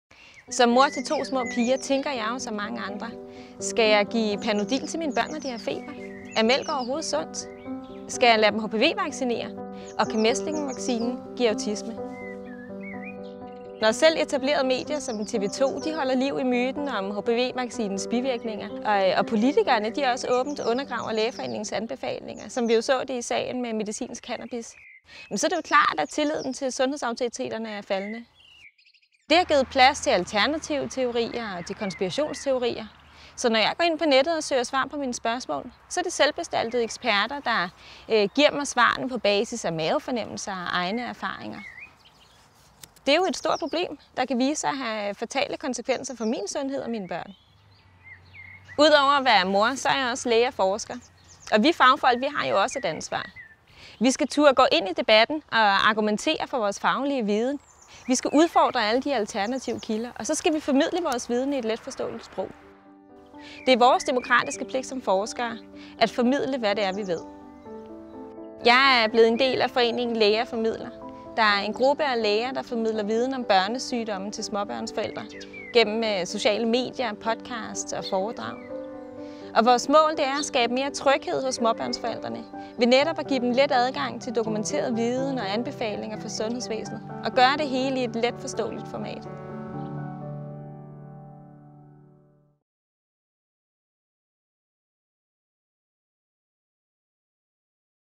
Ida Donkin – forsker i formidling af lægefaglig viden
Ida Donkin var finalist i Ph.d Cup i 2016.

Filmen er produceret af Thin Green Line Productions.